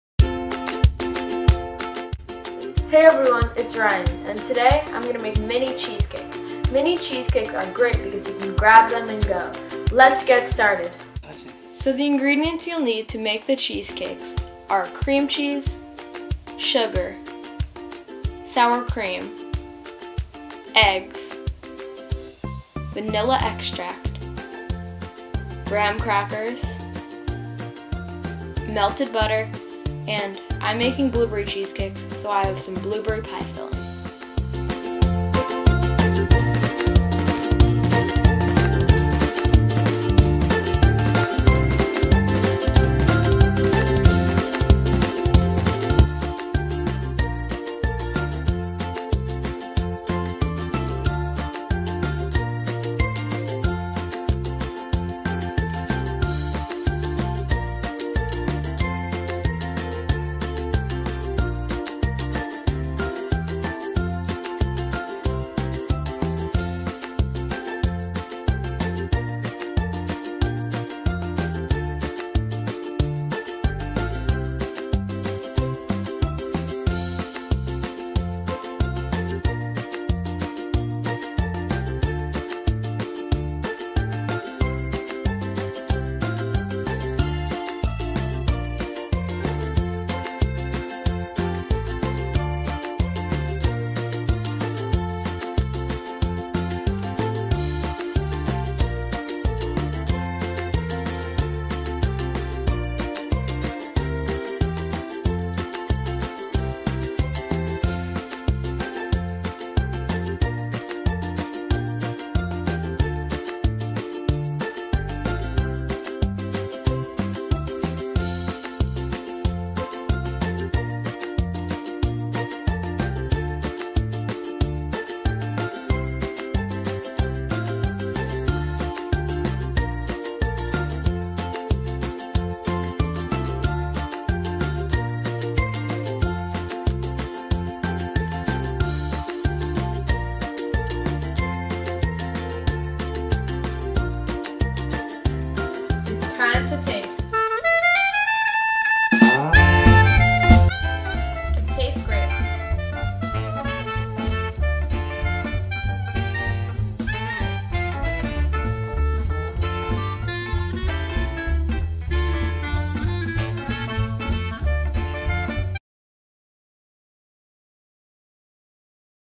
How to Make Mini Cheesecakes I Episode 4 Baking with Ryan
Ever want to make a cheesecake without all the fuss? Kid chef Ryan Wilson has the perfect recipe for you! Easy to make and delicious to eat!

Ingredients:
2 (8 oz) blocks of cream cheese
1/4 cup sour cream
2 eggs
1/2 cup sugar
5 graham crackers
3 tbsp melted butter
1 tsp vanilla extract
Fruit topping (optional)